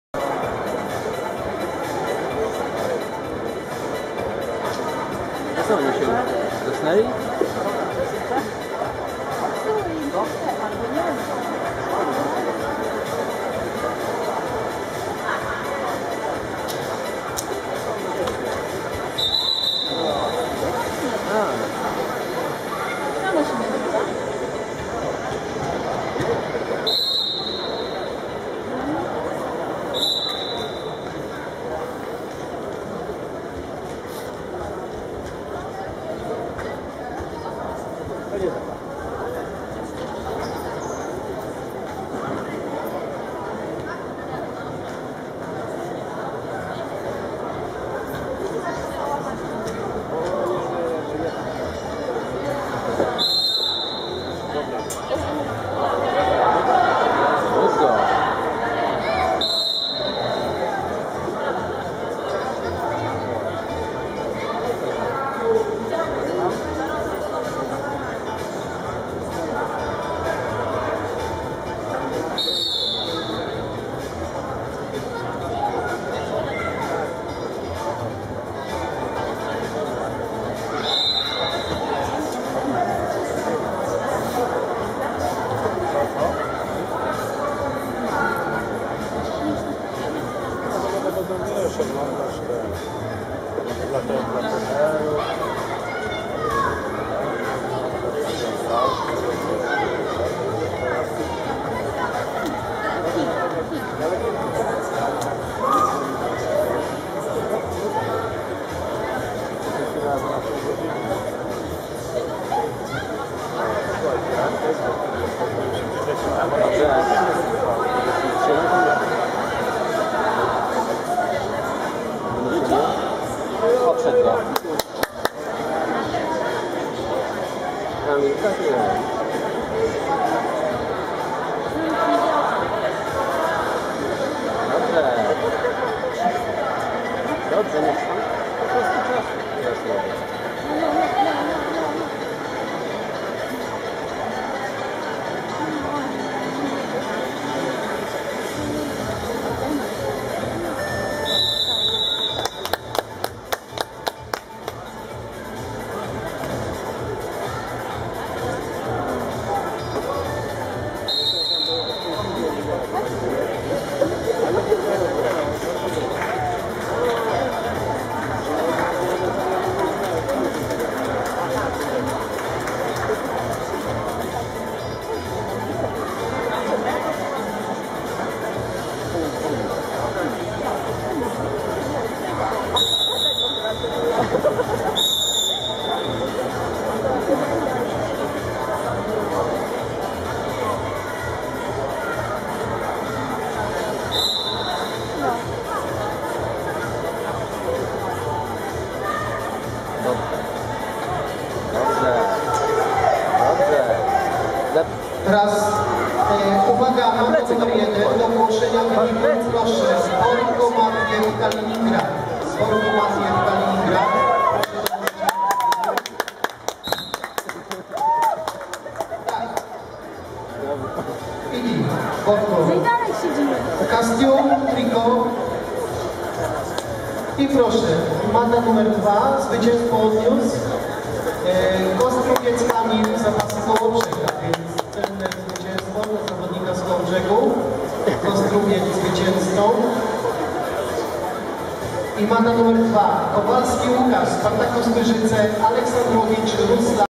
Kostrubiec vs. Nalewajko.mts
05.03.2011 II Puchar Polski Seniorów Białogard.Kamil w stroju niebieskim.